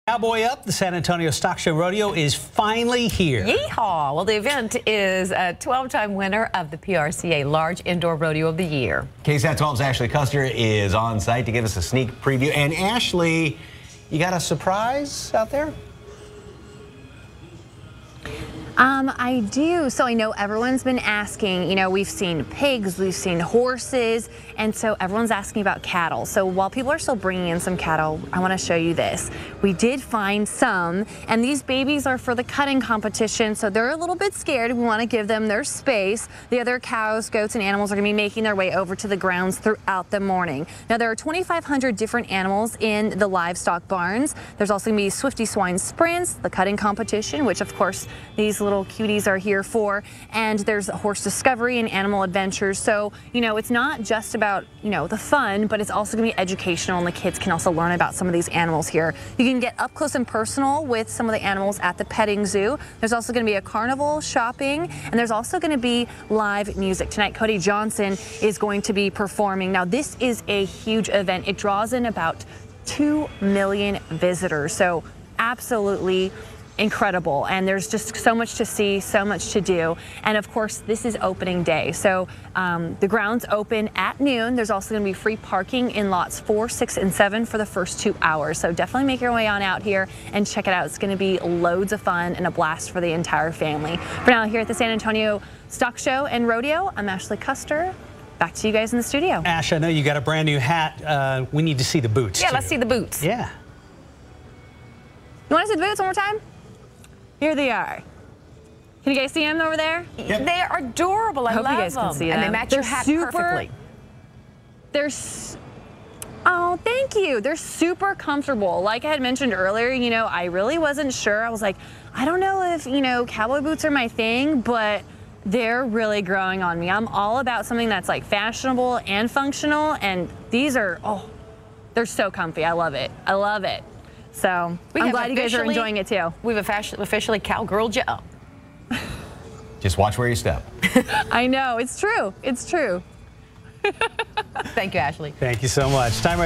KSAT 12's Ashly Custer shows livestock from San Antonio Stock Show & Rodeo
KSAT 12's Ashly Custer shows off her cowboy boots and the livestock at the San Antonio Stock Show & Rodeo.